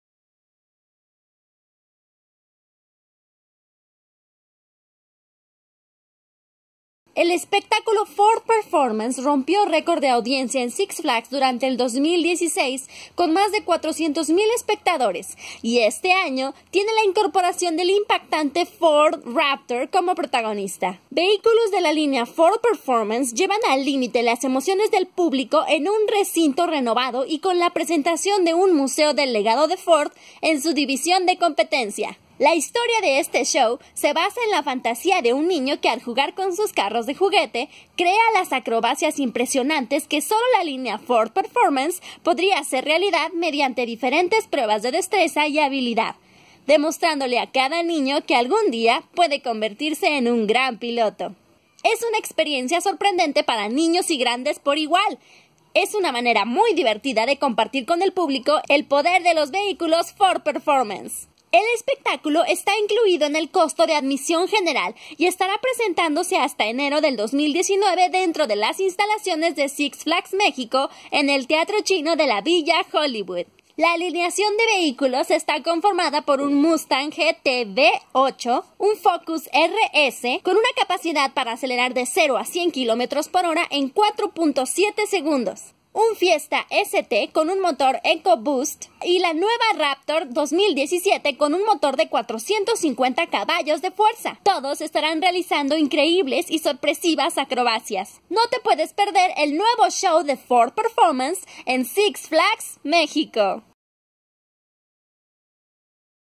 El Show Ford Performance en su segunda edición en SIX FLAGS MÉXICO
El show que rompió récord de audiencia esta de regreso con nuevas e increíbles acrobacias no te lo puedes perder!!!